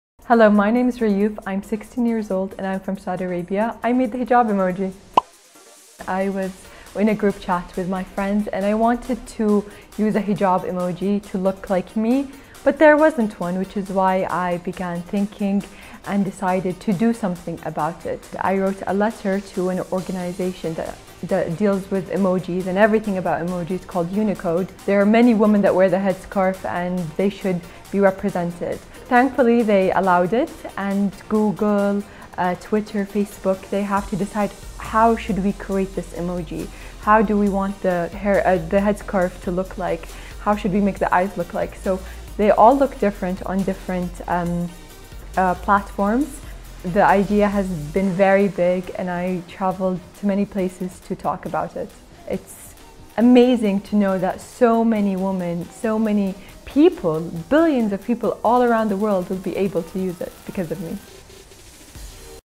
'I designed the hijab emoji'
When 16 year-old Rayouf from Saudi Arabia couldn't find an official emoji that looked like her, she decided to create one herself.